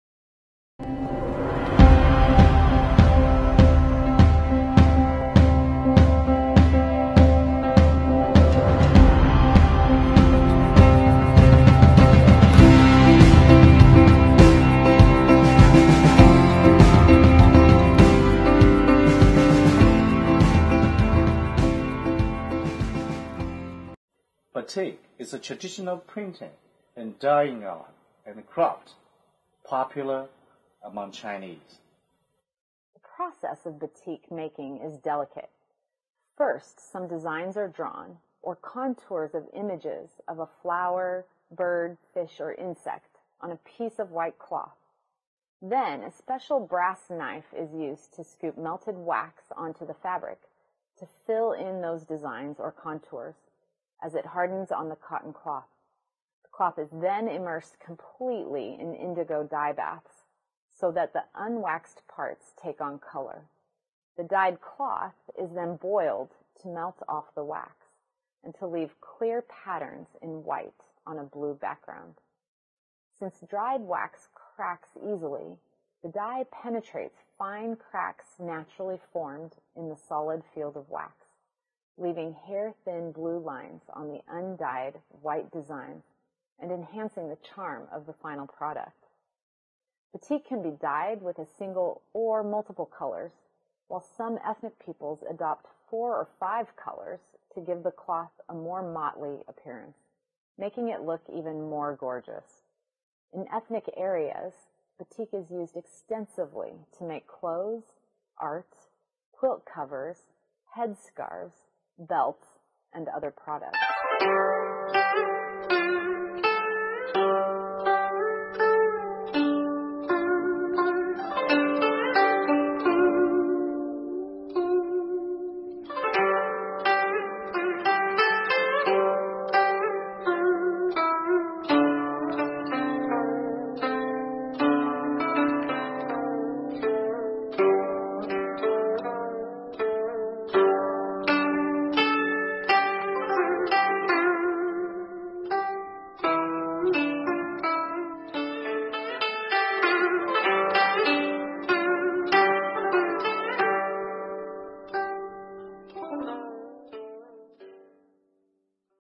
Batik -- Distinct features of Chinese Costume
Batik is a traditional printing and dyeing art and craft popular among Chinese.  In this part, you will know what is Batik and the process of making Batik. Let's learn something about this unique craftsmanship!!